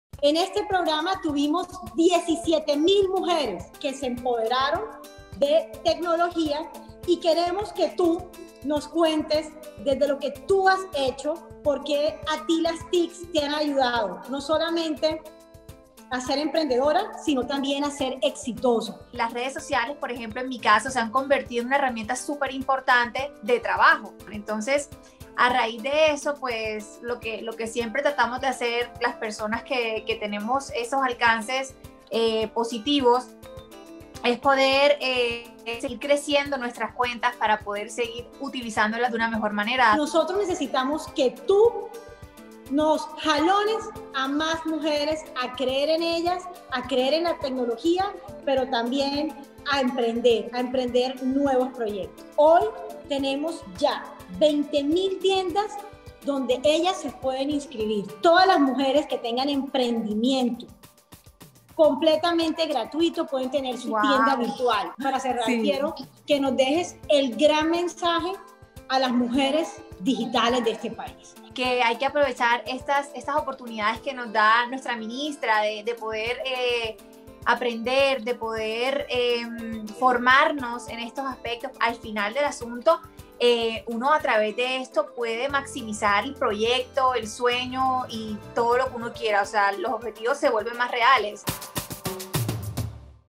Por TIC Mujer y la cruzada por promover el emprendimiento de la mujer colombiana
En 2020 Por TIC Mujer logró certificar a 17.500 colombianas en competencias digitales, una iniciativa que promovió el desarrollo de emprendimientos haciendo uso de las tecnologías como un mecanismo de generación de ingresos y de prevención de violencias.